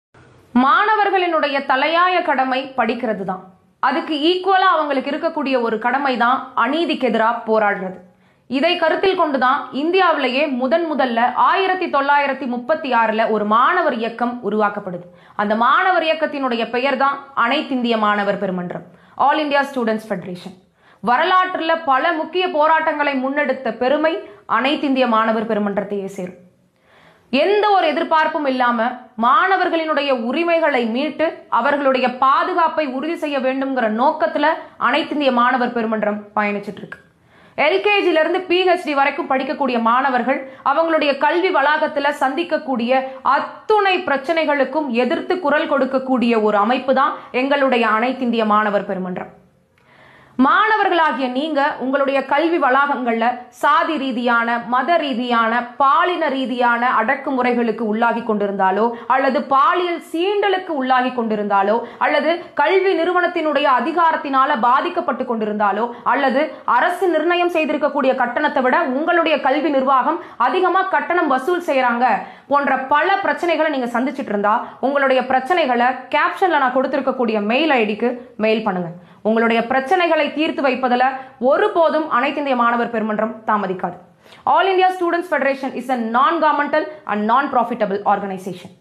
Tamilnadu All India Students Federation !
எந்த ஒரு எதிர்பார்ப்பும் இல்லாமல் முழுக்க முழுக்க மாணவர்களின் உரிமைகளை மீட்டு அவர்களுடை பிரச்சனைகளை எந்த  மாணவராக இருந்தாலும் இவர்களிடம் தெரிவிக்கலாம் காணொளியை முழுசாக காணவும்!